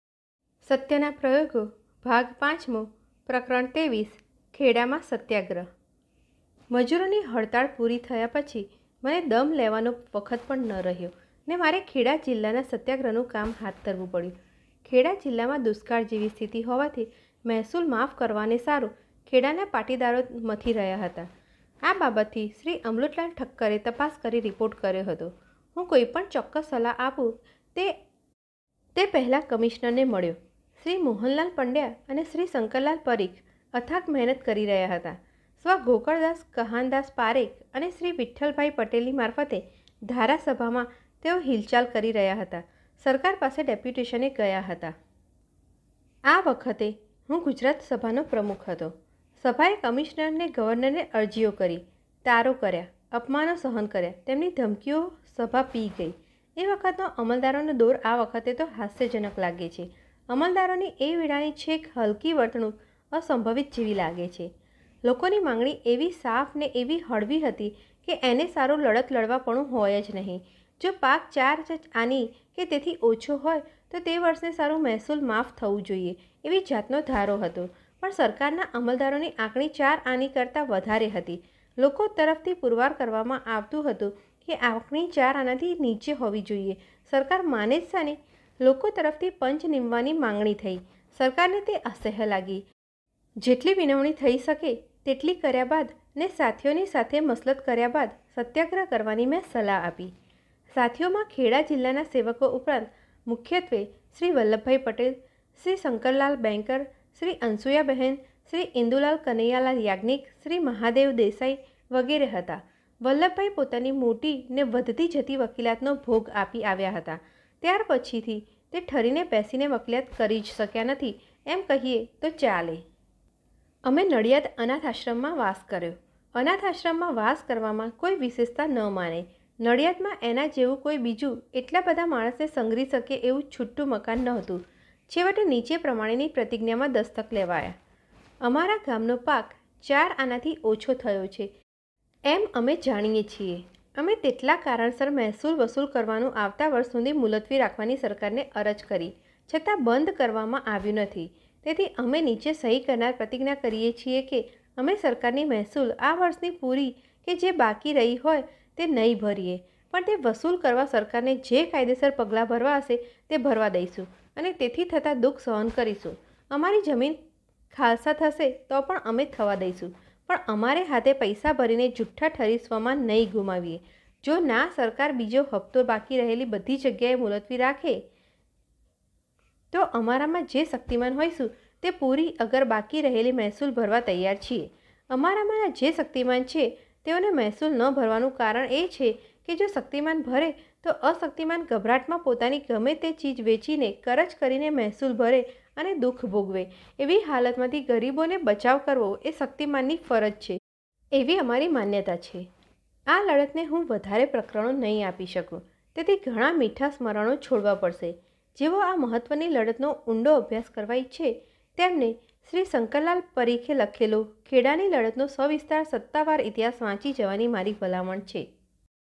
Part 5.23- My Experiments with Truth -SatyanaPrayogo -સત્યનાપ્રયોગો -ભાગ પાંચમો -23.ખેડામાંસત્યાગ્રહ
Part 5.23- My Experiments with Truth - Satyana Prayogo - સત્યના પ્રયોગો - ભાગ પાંચમો - 23.ખેડામાં સત્યાગ્રહ

An Autobiography : The story of My Experiments with Truth of Mahatma Gandhi (Mohandas K. Gandhi)

Audio Biography of Mahatma Gandhi.

'Experiments of Truth' In this book, Gandhiji has told his own stories and life stories on his life. The autobiography of his good and bad experiences is told but Gandhiji has given a great guidance by telling his stories in this book. His life is an inspiration. In this book, Gandhiji has said that I do not want anyone to consider my articles as standard. That is my request. I wish all the experiments to be done as much as possible and as much as possible by considering the experiments shown in it as examples.

જીવનચરિત્ર: મહાત્મા ગાંધી (મોહનદાસ કે. ગાંધી) ના સત્યના પ્રયોગોની વાર્તા

મહાત્મા ગાંધીની ઓડિયો જીવનચરિત્ર.

'સત્યના પ્રયોગો' આ પુસ્તક માં ગાંધીજી ના જીવન ઉપર પોતે જ પોતાની વાતો અને જીવન કથા કહી છે. તેમના સારા-ખરાબ અનુભવોની આત્મકથા કહી છે પણ ગાંધીજી પોતાની વાતો આ પુસ્તક માં કહી ને એક મોટું માર્ગદર્શન આપ્યું છે તેમનું જીવન તો એક પ્રેરણાદાયી છે. આ પુસ્તક માં ગાંધીજી એ કહયું છે કે મારા લેખોને કોઇ પ્રમાણભૂત ન ગણે એમ હું ઈચ્છું છું. એવી મારી વિનંતી છે. તેમાં દર્શાવેલા પ્રયોગોને દૃષ્ટાન્તરૂપે ગણીને સહુ પોતપોતાના પ્રયોગ યથાશક્તિ અને યથામતિ કરે એટલી જ મારી ઈચ્છા છે.